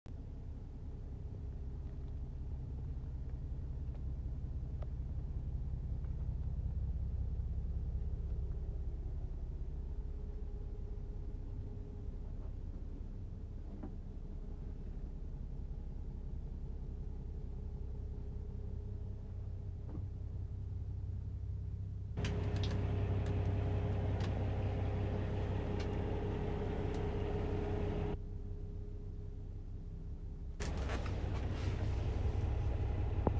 2014 International Prostar Sleeper. Interior
2014 International Prostar Sleeper. Cummins ISX15 engine. New steering wheel.